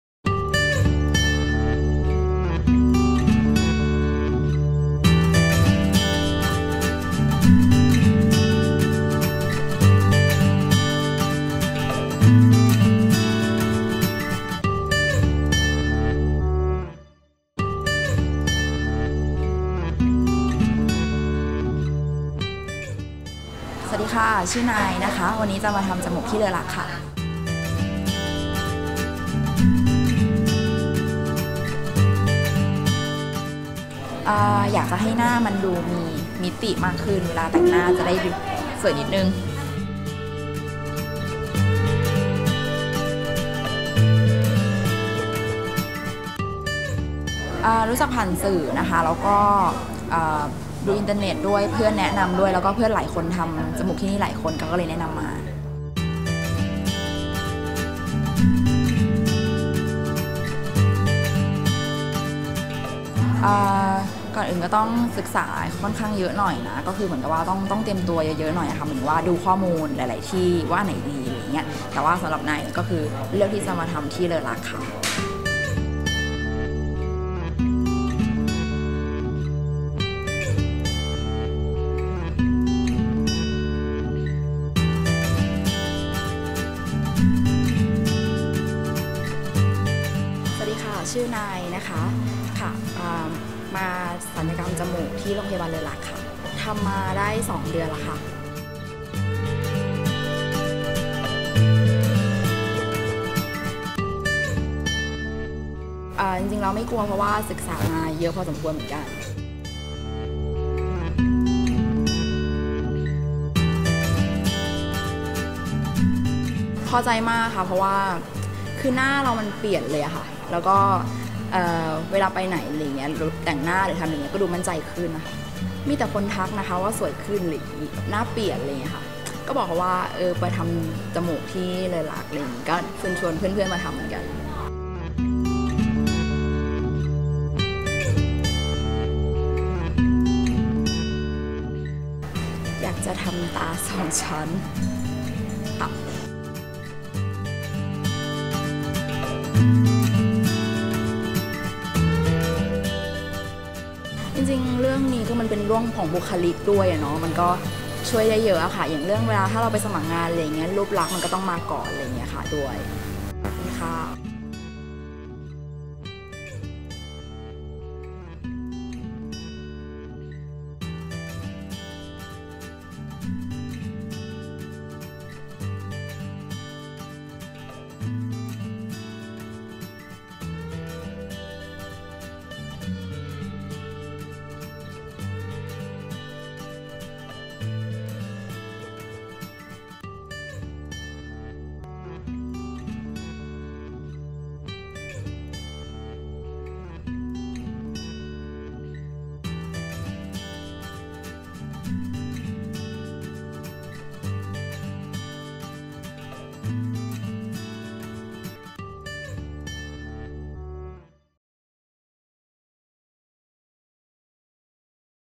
ความประทับใจจากลูกค้าที่มาใช้บริการโรงพยาบาลเลอลักษณ์ (เสริมจมูก)
อีกเสียงความประทับใจจากลูกค้าที่มาใช้บริการโรงพยาบาลเลอลักษณ์
(เสริมจมูก) เรามีหมอผู้เชี่ยวชาญทางด้านศัลยกรรมโดยเฉพาะ ให้คุณ สวย หล่อ ดูดี และปลอดภัยในโรงพยาบาลที่มีมาตฐาน  callcenter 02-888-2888